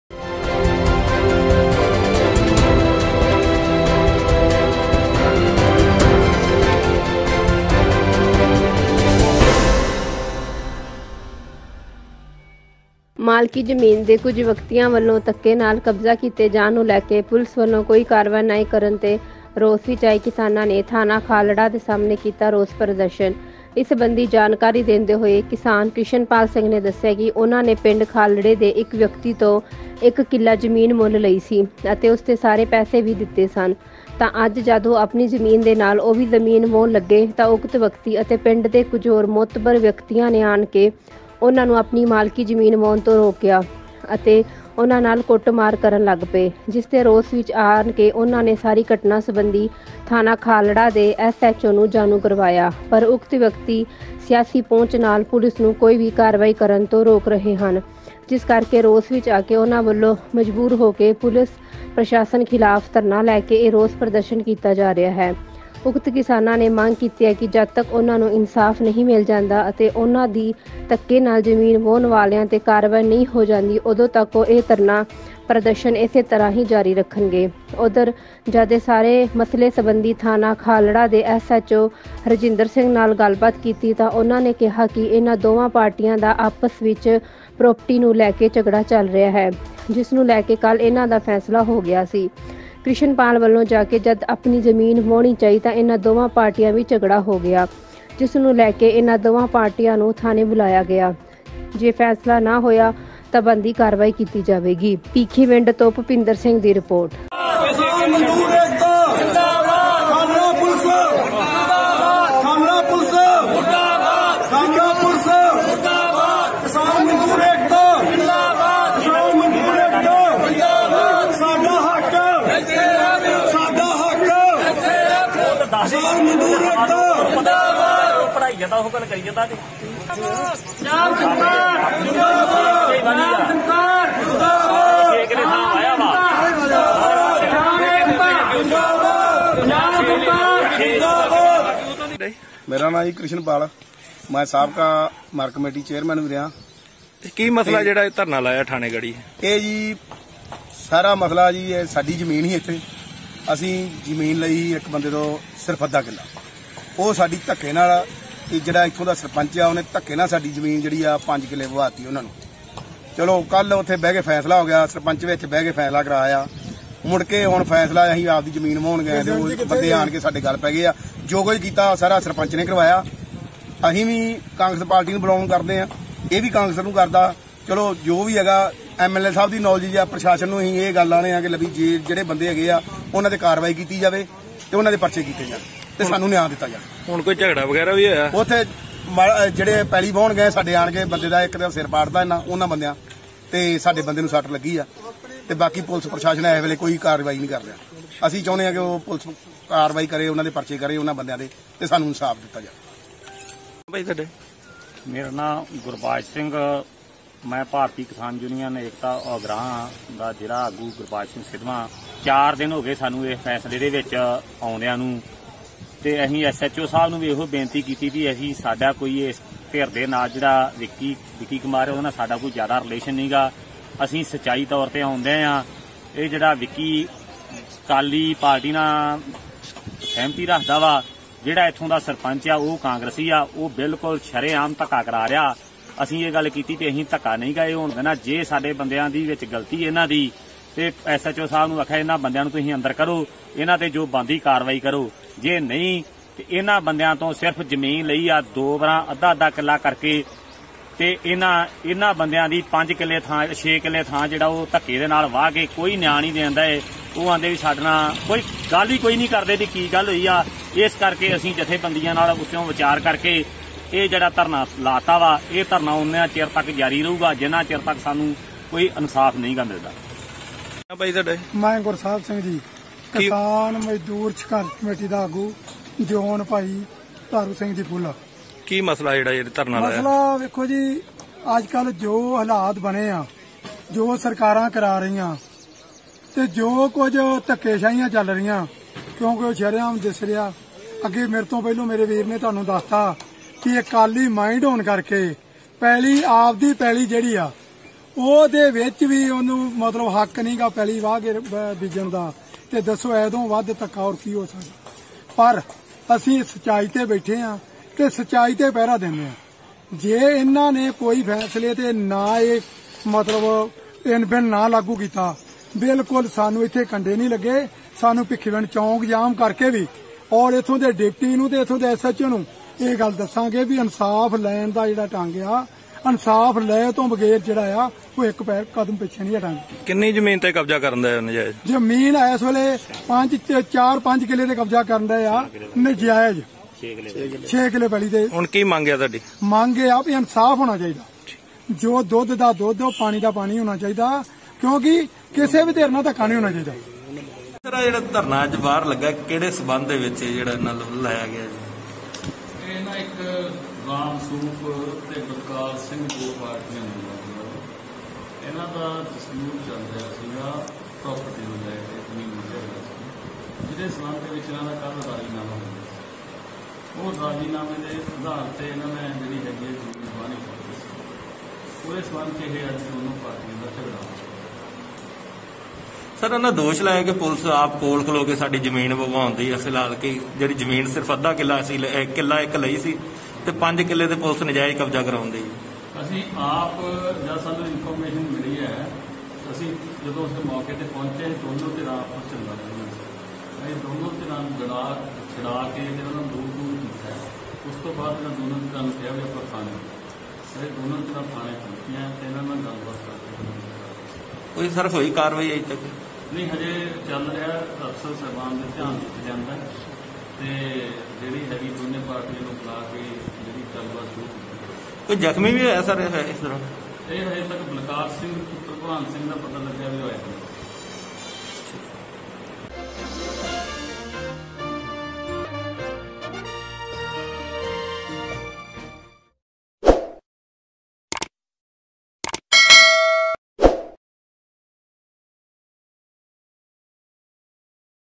ਮਾਲਕੀ ਜ਼ਮੀਨ ਤੇ ਕੁਝ ਵਿਅਕਤੀਆਂ ਵੱਲੋਂ ਧੱਕੇ ਨਾਲ ਕਬਜ਼ਾ ਕੀਤੇ ਜਾਣ ਨੂੰ ਲੈ ਕੇ ਕਿਸਾਨਾਂ ਨੇ ਕੀਤਾ ਰੋਸ ਪ੍ਰਦਰਸ਼ਨ
ਮਾਲਕੀ ਜ਼ਮੀਨ ਤੇ ਕੁਝ ਵਿਅਕਤੀਆਂ ਵੱਲੋਂ ਧੱਕੇ ਨਾਲ ਕਬਜ਼ਾ ਕੀਤੇ ਜਾਣ ਨੂੰ ਲੈ ਕੇ ਪੁਲਸ ਵੱਲੋਂ ਕੋਈ ਕਾਰਵਾਈ ਨਾ ਕਰਨ ਦੇ ਰੋਸ ਵਿੱਚ ਆਏ ਕਿਸਾਨਾਂ ਨੇ ਥਾਣਾ ਖਾਲੜਾ ਦੇ ਸਾਹਮਣੇ ਕੀਤਾ ਰੋਸ ਪ੍ਰਦਰਸ਼ਨ